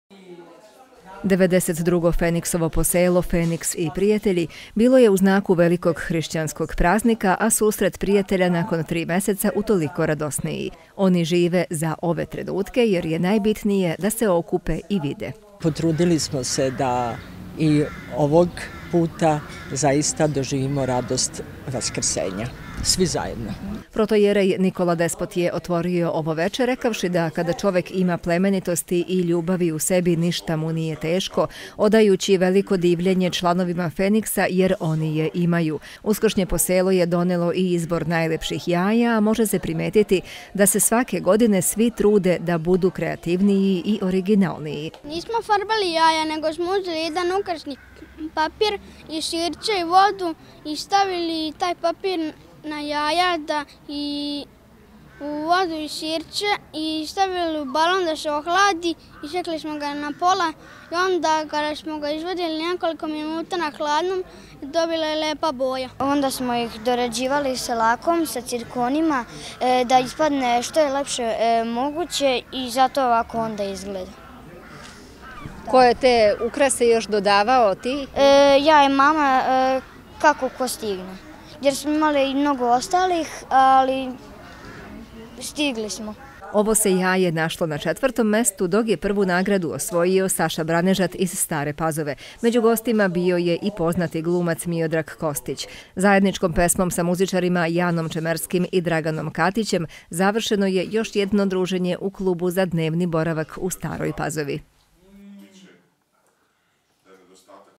Uskršnje Feniksovo poselo
Nakon tromesečne pauze, održano je Feniksovo poselo Feniks i prijatelji, kada su se okupili članovi i prijatelji ovog Društva, da zajedno proslave Uskrs i da se druže. Ujedno je organizovan izbor za najlepše vaskršnje jaje, pri čemu je bilo primetno da ova akcija uvek privlači pažnju I svake godine sve je više kreativnosti kod članova I prijatelja.